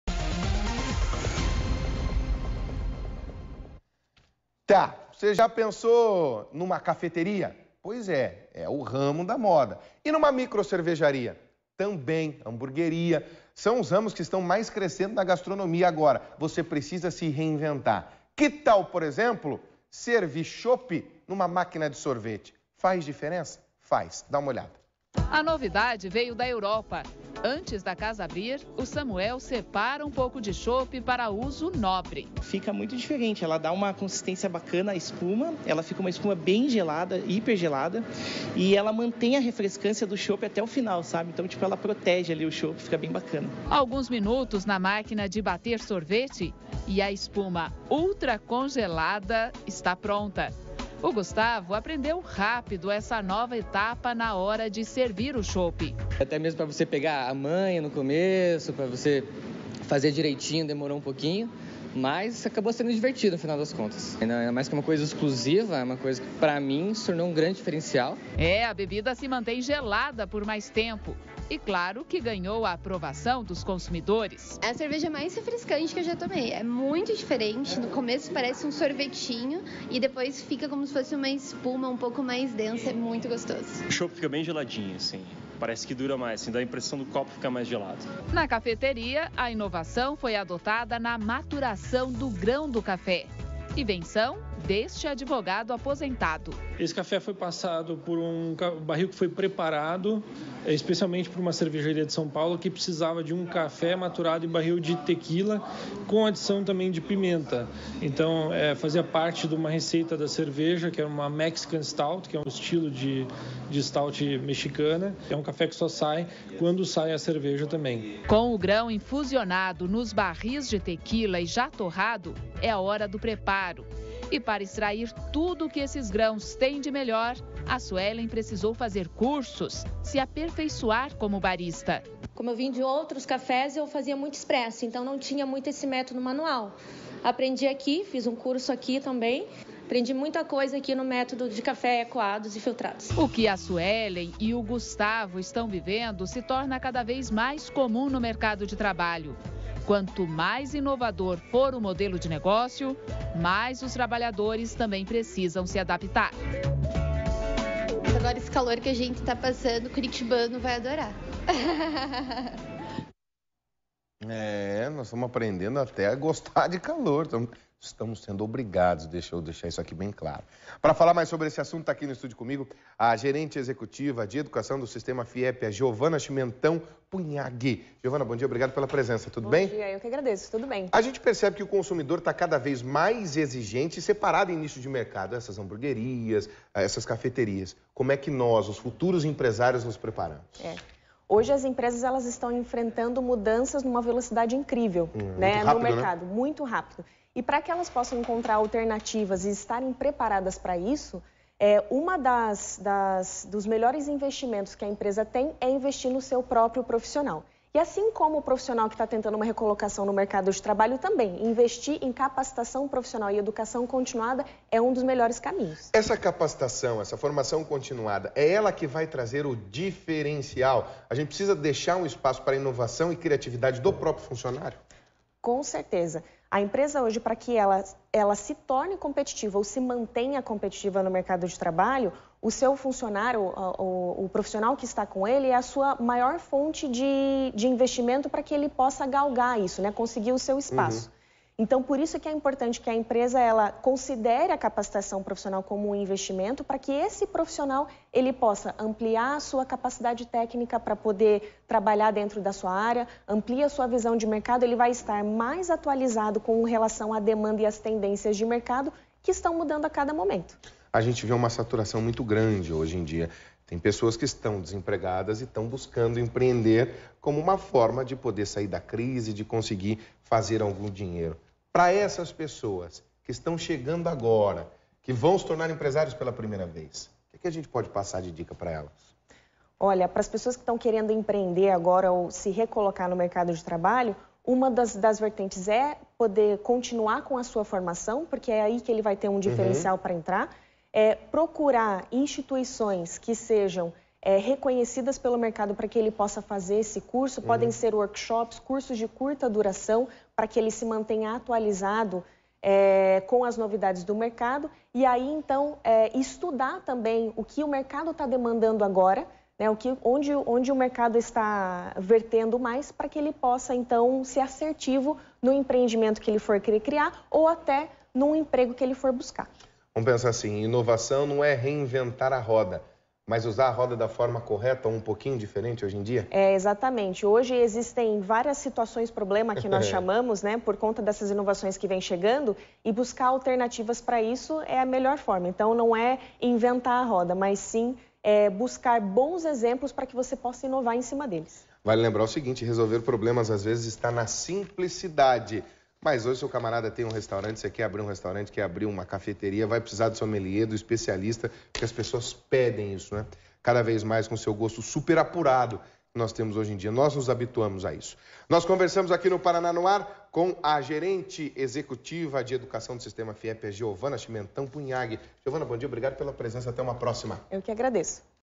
Emprego: Bebidas artesanais viram tendência e fazem profissionais se especializarem
Quando falamos de gastronomia, sempre existe um novo modelo de negócio que está “na moda”. Esse é o caso das hamburguerias e “iogurterias”. Hoje, a tendência fica por conta das cervejarias artesanais e das cafeterias. Com isso, muitos profissionais estão se especializando no assunto. A Gerente Executiva de educação do sistema FIEP, Giovana Chimentão Punhagui fala mais sobre esse assunto. Confira!